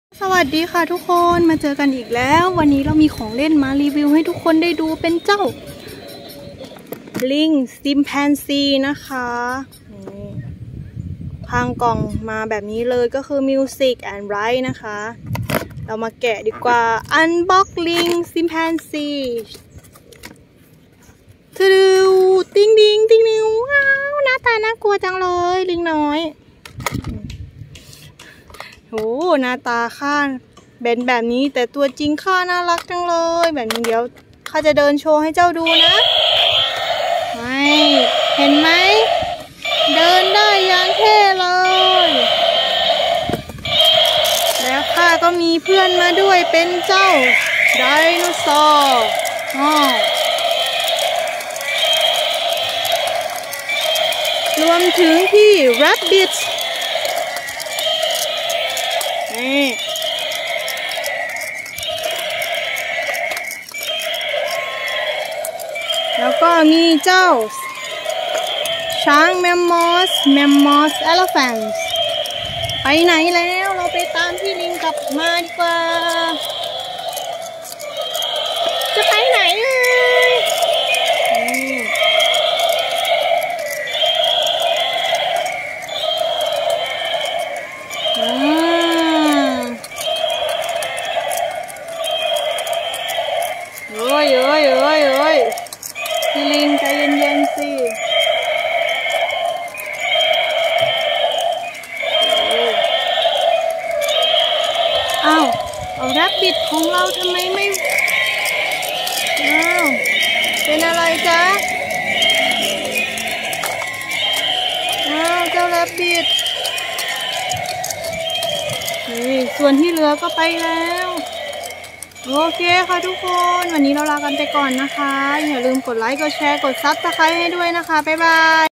ีรีวิว แกะกล่องลิงชิมแปนซี สีดำ มีช้างและไดโนเสาร์ด้วย
Review: Unboxing black chimpanzees, elephants and dinosaurs.